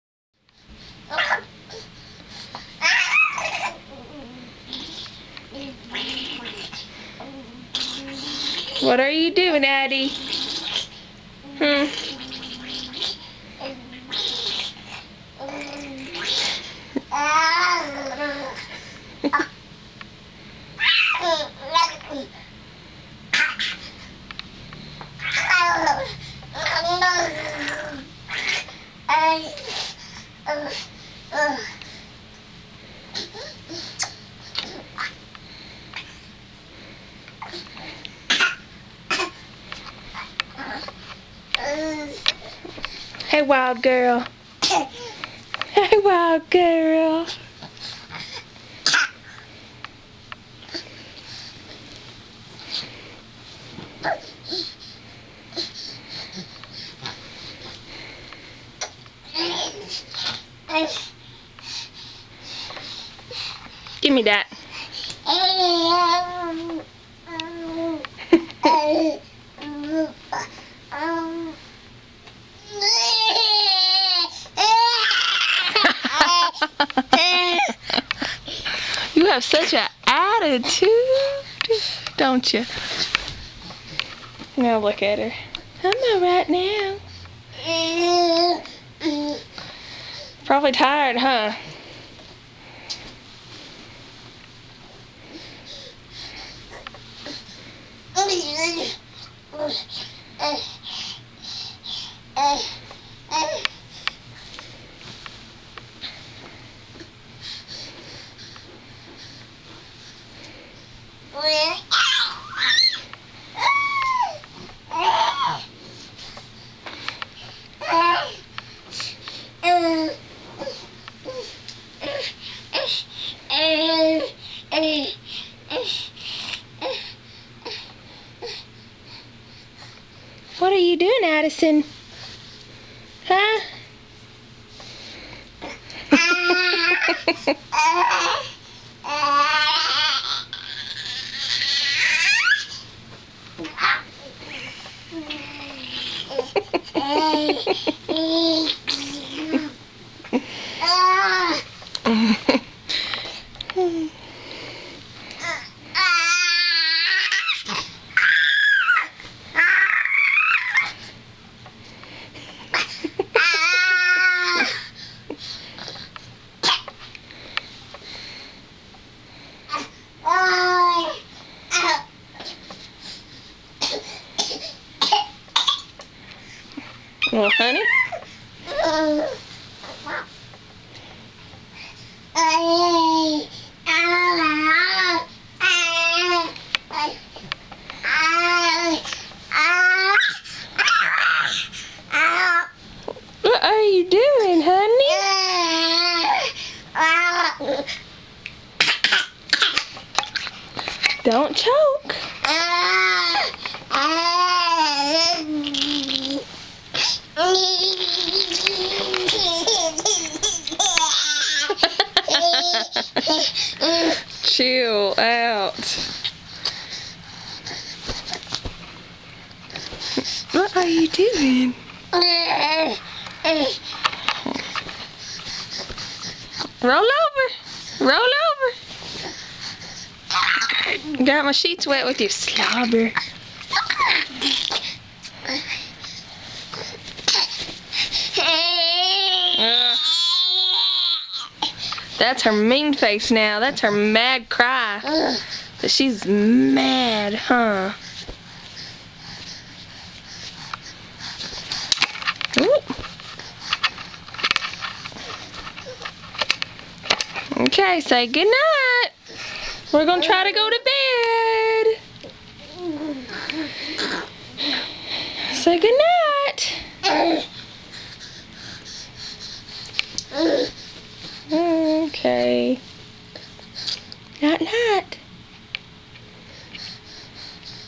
addi rollig on bed
addison played on the bed for a whole hour rolling